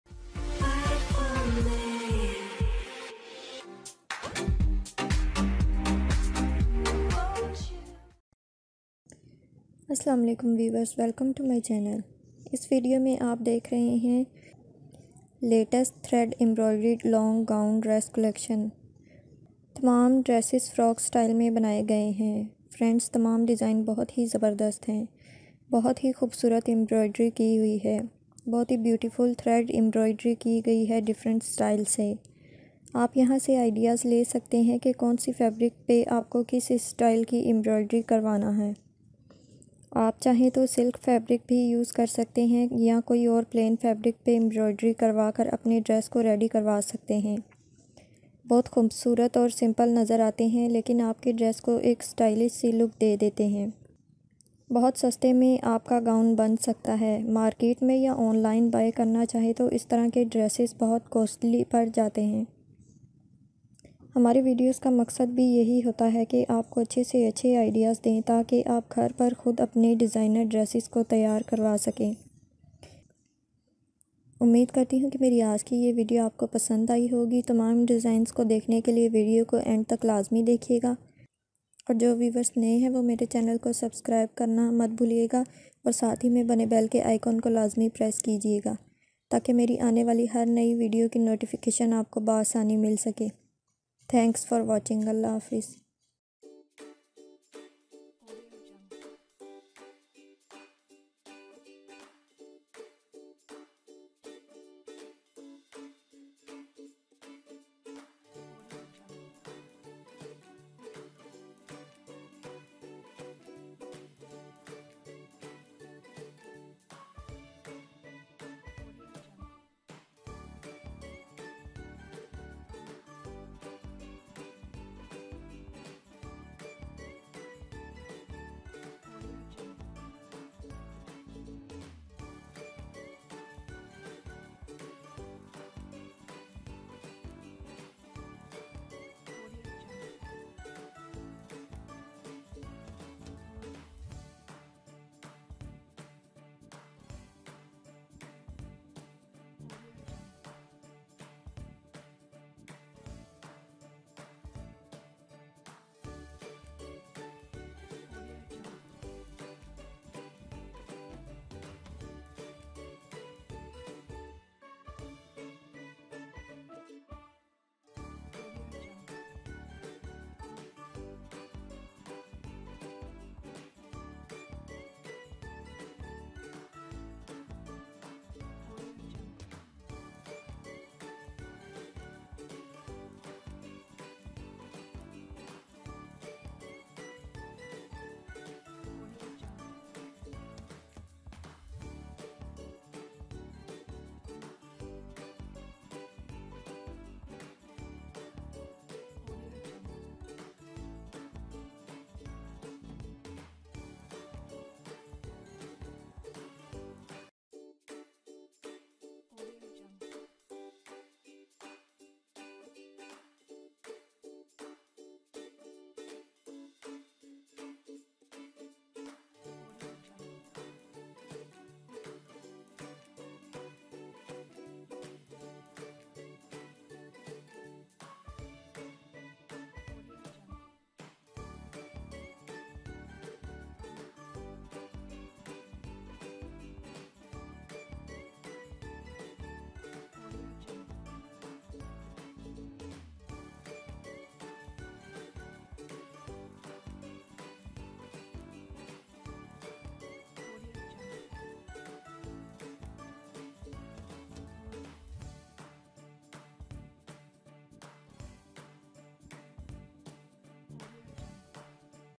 Latest Beautiful And Outstanding Thread Work Embroidery Gown Designs
Hello Friends Welcome To My Channnel .This video Is About Latest Beautiful And Outstanding Thread Work Embroidery Gown Designs
Fashion Stuff.